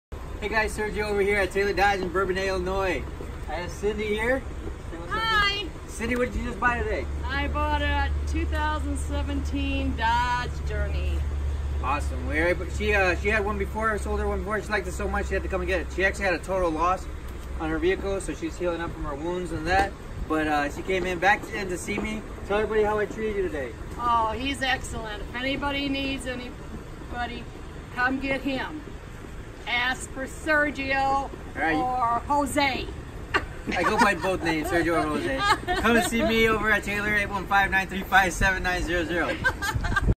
Taylor Chrysler Dodge Reviews: Testimonial by Cynthia about a 2017 dodge Journey
Meet Cynthia Walker, one of the many happy car buyers who bought a 2017 dodge Journey at Taylor Chrysler Dodge.
Make sure you stop by Taylor Chrysler Dodge today to take advantage of our huge inventory of new and used vehicles.